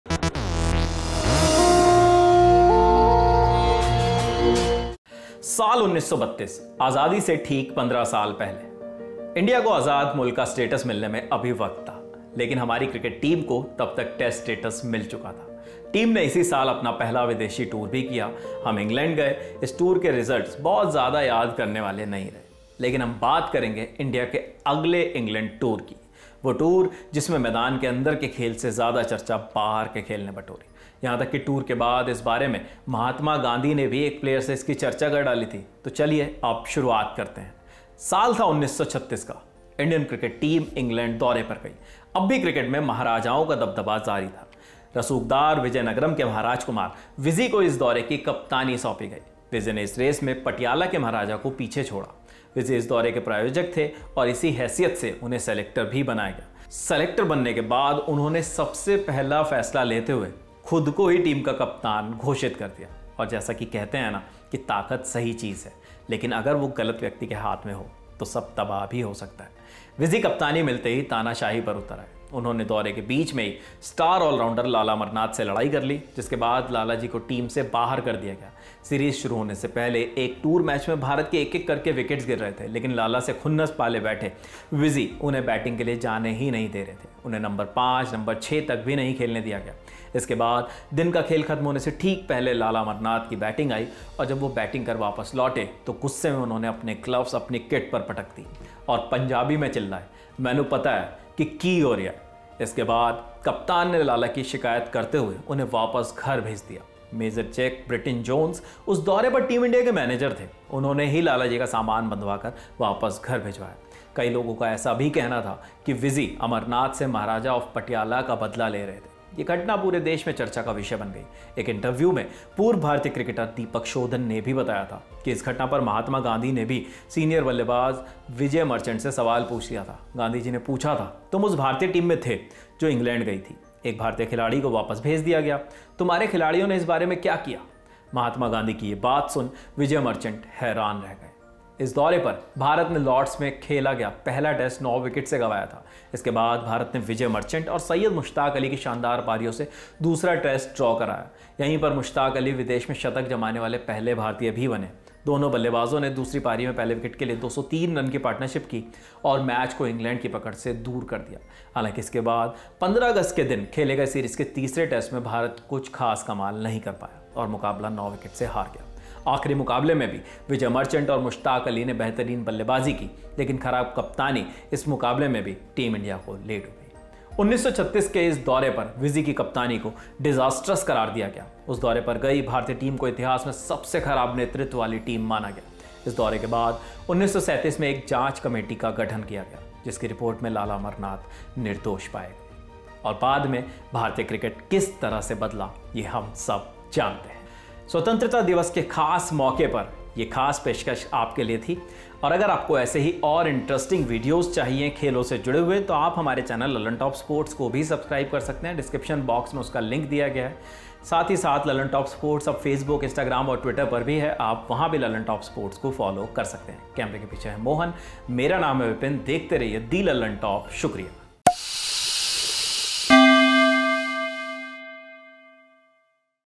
IND vs ENG 1936 Tour की कहानी, जब Lala Amarnath को दौरे से वापस भेज दिया गया था | Mahatma Gandhi
IND vs ENG 1936 Tour: We will talk about India's next tour of England. The tour, in which the 'game' outside got more discussion than the game inside the field. Even Mahatma Gandhi had discussed it with a player after the tour. So let's start now. Year 1936. The Indian cricket team went on a tour of England. Even now the dominance of Maharajas in cricket continued. Maharajkumar Vizi of Vizianagaram was given the captaincy of this tour. Vizi had left behind the Maharaja of Patiala in this race. Viji was the sponsor of this tour and in this capacity he was made a selector. After becoming the selector, he took the first decision and declared himself the captain of the team.